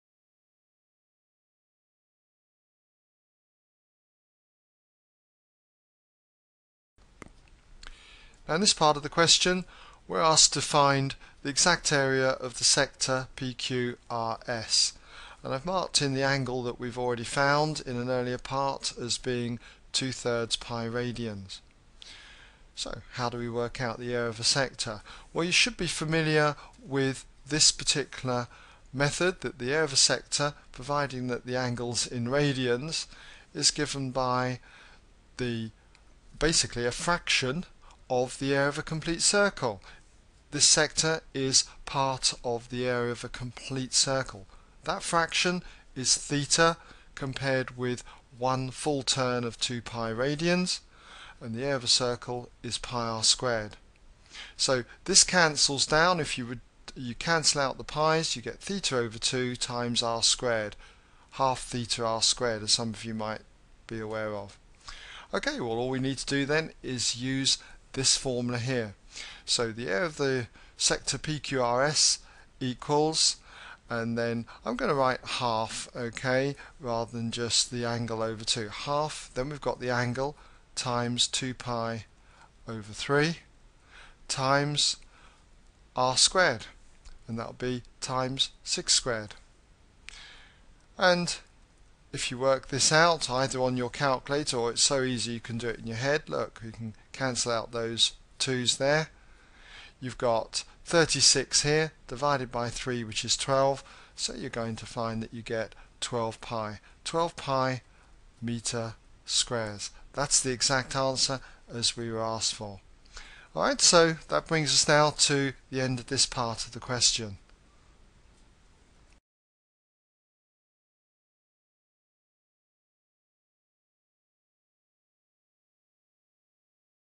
A-Level Maths Edexcel C2 January 2007 Q9(b) : ExamSolutions
Worked solution to the above Core 2 question on finding the area of a sector where the anle is in radians. To see the question goto ExamSolutions